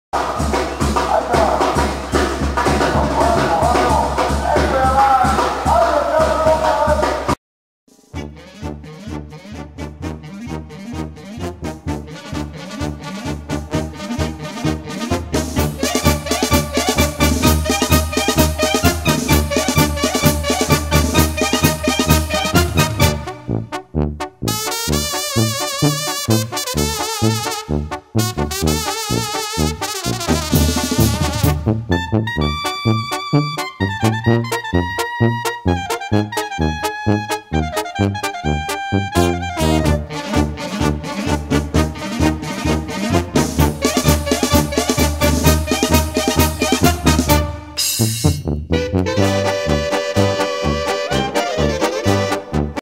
Yo Tratando de poner una Banderilla jeje
Bien señores me hubiera gustado poner las 2 veces que quise intentar poner una banderilla a un toro de raza d lidia y la verdad tenia algo de miedo por el tamaño del ejemplar, pero el gusto es el gusto jeje bueno las banderillas honestamente no son como las profesionales el tamaño del palo y el pico con el que se encaja era mas ancho y el puente mas largo algo casi imposible que le entrara al toro, que por una parte esta bien para no lastimar al toro! Pero como quiera me gane mi botella de cazadores jeje lastima que no me gusta mucho el tekila pero lo comparti con mis compas!
Bueno ahi kedo mi video despues subire unas vueltas que tengo pendientes!